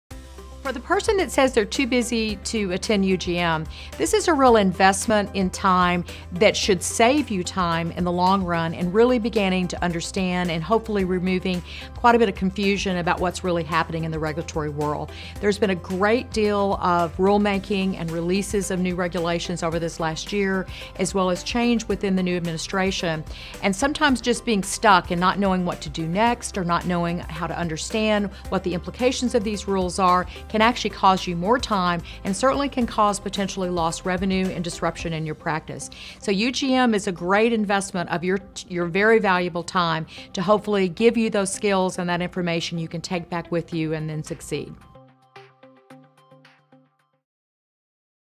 Benefits of Attending UGM | NextGen Healthcare
ugm2018 nextgenhealthcare What will you gain from attending this year’s User Group Meeting (UGM)? Hear Cherie Holmes-Henry, Vice President of Industry Affairs at NextGen Healthcare, share the many reasons why UGM is worth your time.